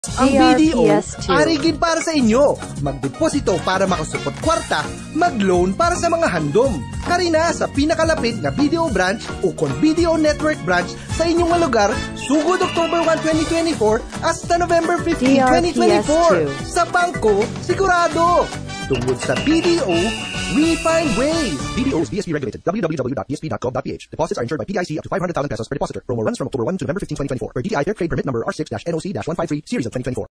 BDO Network Bank Negros Occidental Fiesta Radio Ad Q4 2024 30s [ST]
Ito po ang Q4 2024 Radio Ad ng BDO Network Bank Negros Occidental Fiesta.

DRPSTwo 8968: BDO Network Bank Negros Occidental Fiesta Radio Ad Q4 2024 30s [ST]
DTI FTEB-R6-NOC-153/s.24 PHCA CID PHCA-2024H2-7305/October 29, 2024